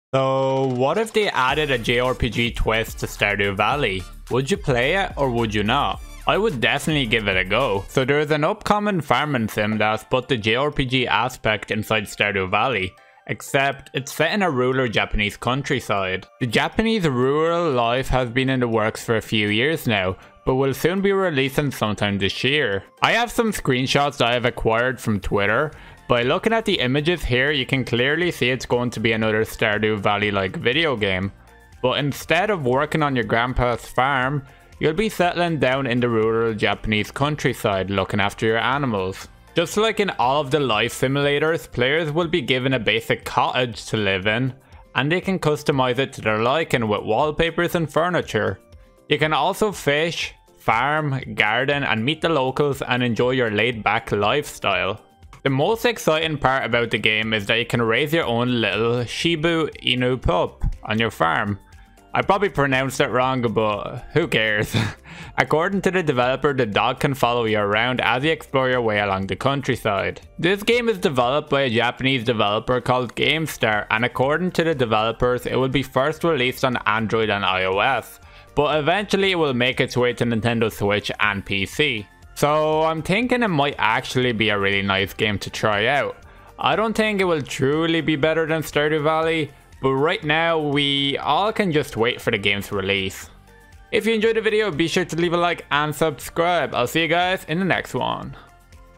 Japan’s Most Wholesome Farming Sim
If you're looking for a Stardew Valley game that's similar to the one you play on your computer, but with a Japanese twist, then look no further! In this video, I'll show you everything i know about the brand new upcoming game called Japanese Rural Life !!

Time Stamps :

0:00 Intro
0:23 A Few Images About The Game !!
0:35 A Little Bit About The Game !
1:15 What consoles will it be released on ?!
1:23 Summary
1:34 Outro